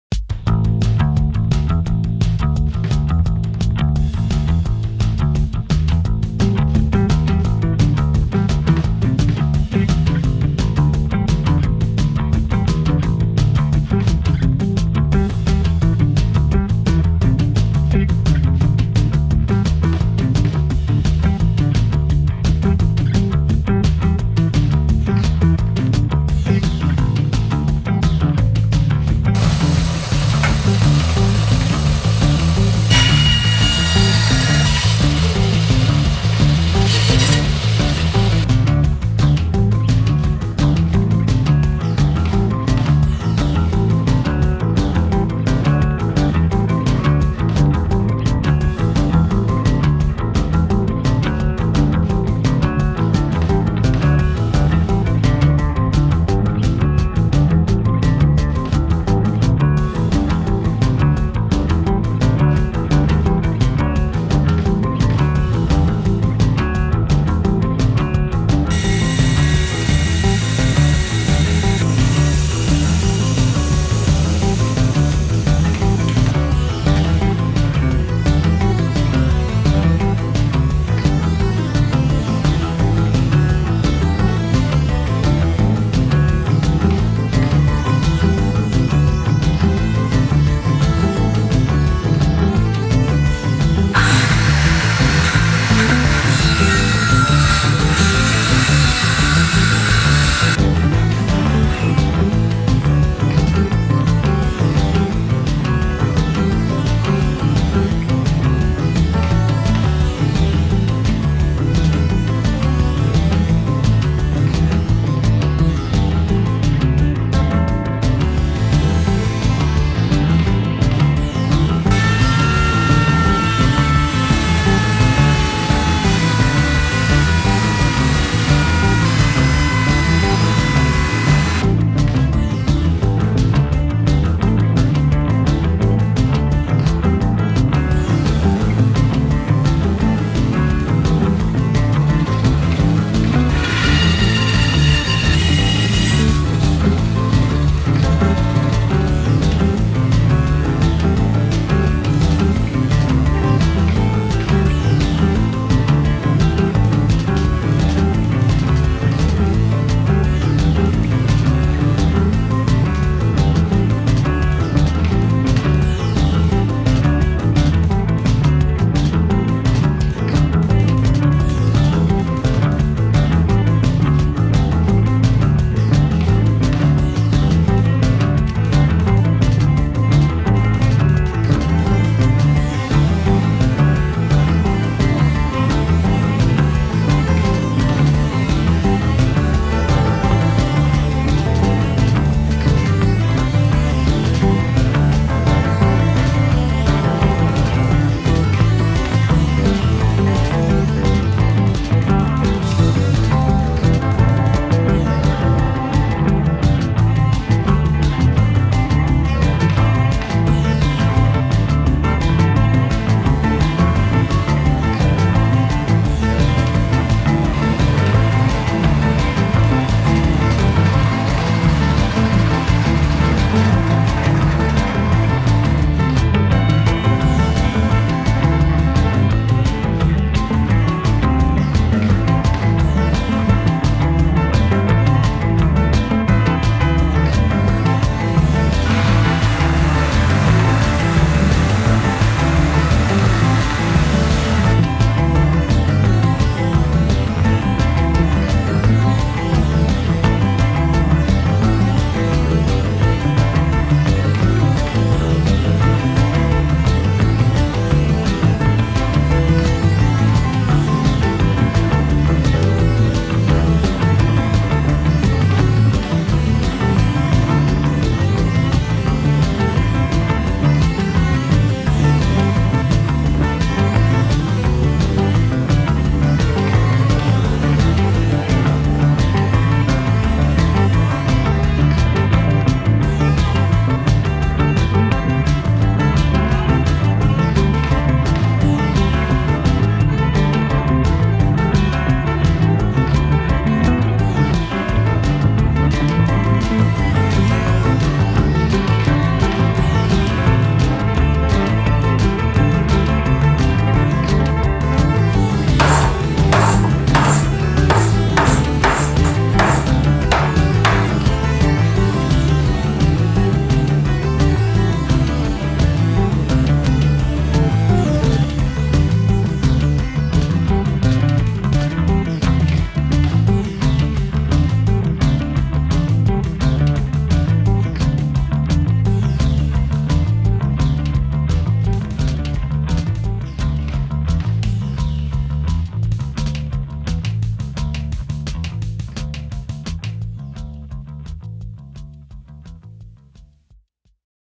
Wood & Leather Chisel Box || DIY
I like to keep my tools clean and organized, so I made a wooden box to store my chisels.

I like how the wood and leather mix together and, even if I used plywood for the top and bottom, after a bit of wood stain it looks really clean :)